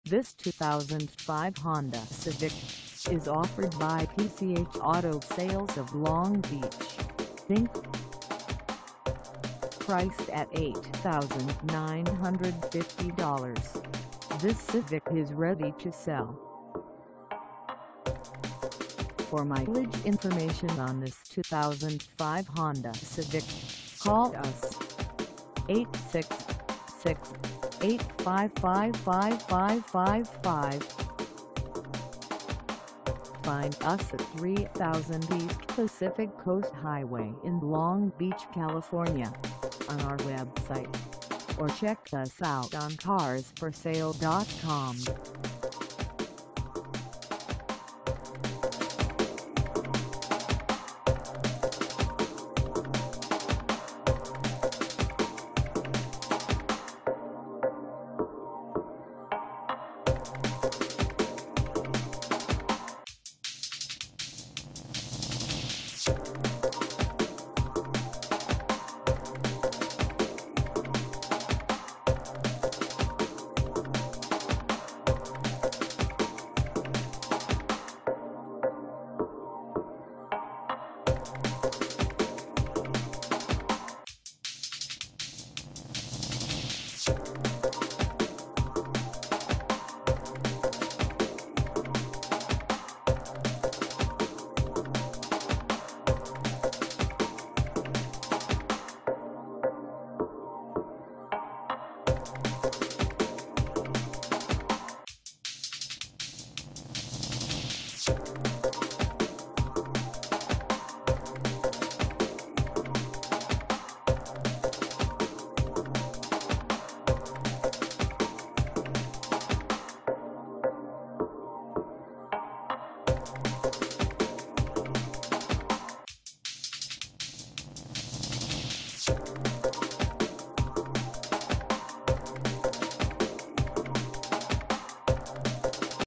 2005 Honda Civic for sale in Long Beach, CA 90804 at the PCH
This 2005 Honda Civic is for sale in Long Beach, CA 90804 at PCH Auto Sales of Long Beach, Inc.
Engine: I4 1.7L I4
Transmission: Manual 5-Speed
Color: BLUE
Mileage: 0
Contact PCH Auto Sales of Long Beach, Inc 3000 East Pacific Coast Highway Long Beach, CA 90804 to test drive this 2005 Honda Civic  today.